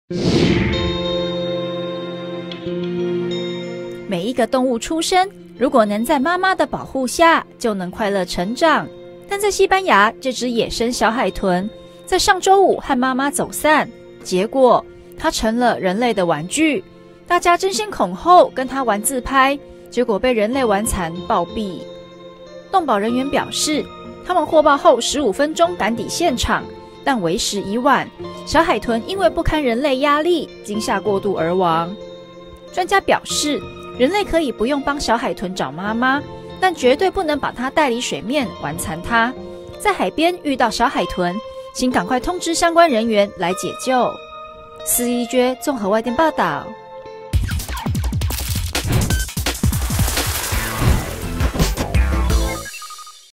可憐小海豚 被人類玩殘暴斃慘死 | 台灣蘋果日報
又傳出有小海豚被人類玩殘暴斃的事件，這次發生在西班牙莫哈卡爾(Mojacar)的海邊，待動保人員前來拯救這隻小海豚時，小海豚已經不幸身亡。
據英國《每日郵報》報導，事發在上周五，這隻小海豚疑似與媽媽走失，隨後在海邊被人類發現，許多民眾抱著好奇心，紛紛抱著或撫摸小海豚玩自拍。但果然好奇心能殺死一隻小海豚，動保人員表示，他們在接獲報案後15分鐘趕到現場，但卻發現小海豚已經死亡。動保人員也指出，小海豚是因為太多人類靠近牠，加上媽媽不在身邊，導致牠壓力過大與緊張而死亡。